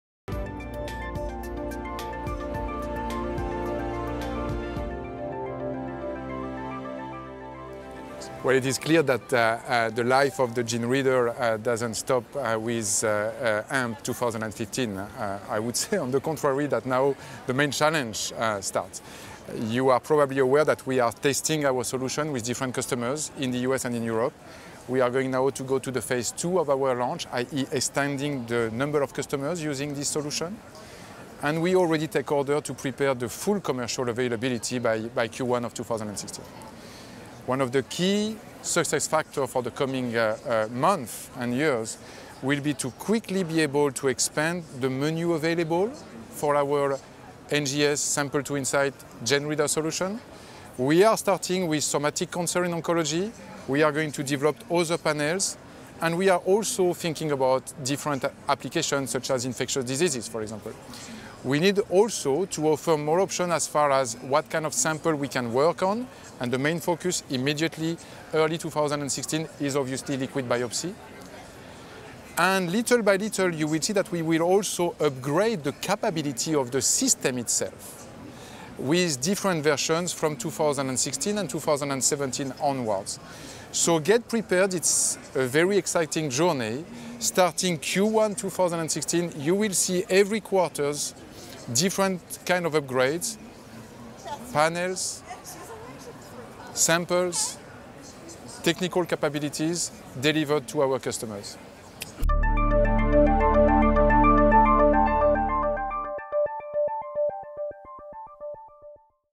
Interview with Thierry Bernard, Head of Molecular Diagnostics Business Area, QIAGEN
Thierry Bernard, Senior Vice President, Molecular Diagnostics Business Area at QIAGEN, on the future plans for the GeneReader NGS System